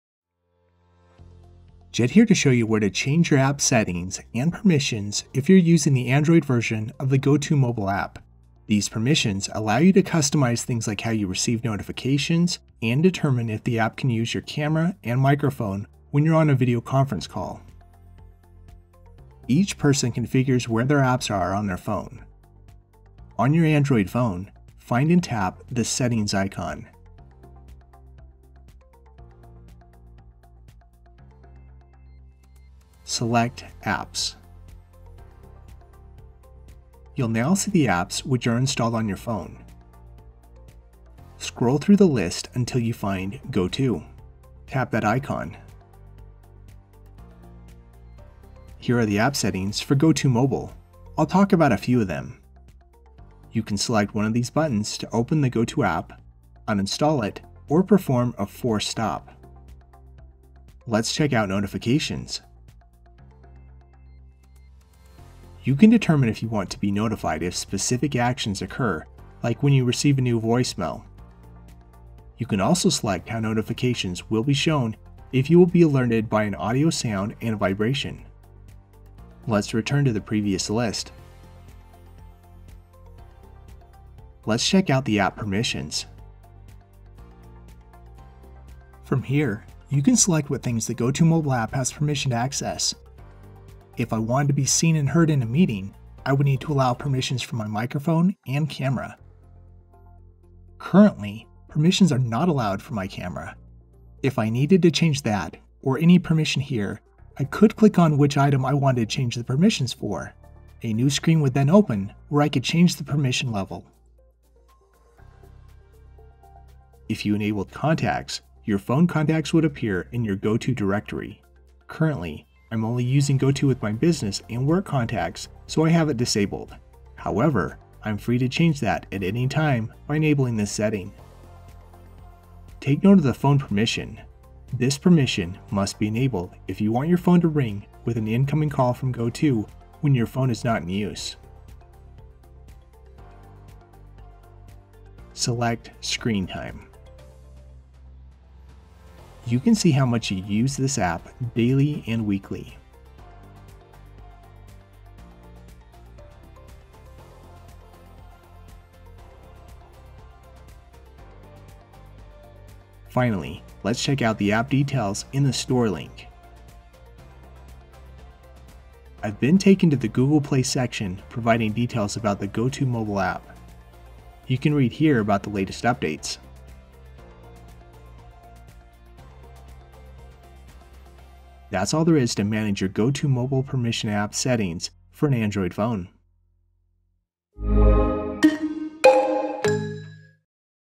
Android Settings for the GoTo Mobile App
This video demonstrates where to find your Android phone's GoTo Mobile app settings. These settings allow you to decide on the permission levels for things like your microphone and camera.

0:00 Introduction
0:22 Locate Settings icon and then Apps
0:36 Locate the GoTo mobile app
0:54 Open, uninstall, and force stop
1:00 Notifications
1:23 Permissions
2:24 Screen time
2:42 App details in store
3:00 End credits